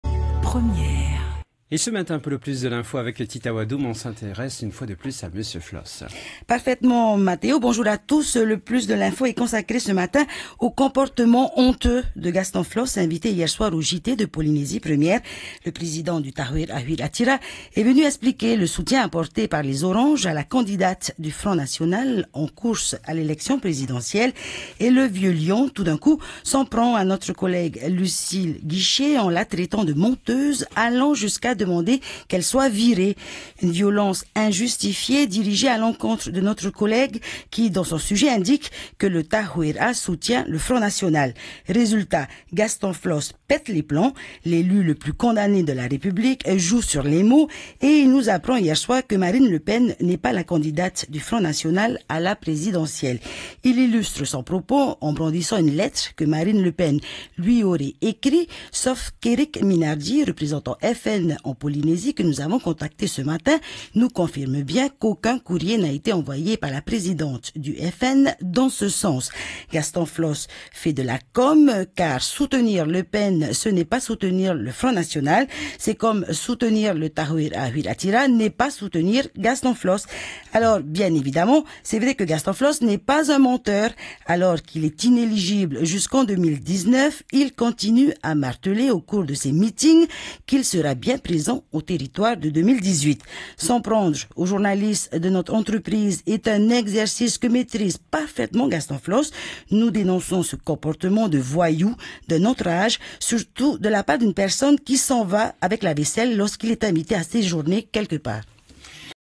Le plus de l'info : Gaston Flosse - 24/03/2017
Le « Plus de l’info » est consacré ce matin au comportement honteux de Gaston Flosse, invité hier soir du JT de Polynésie 1ère.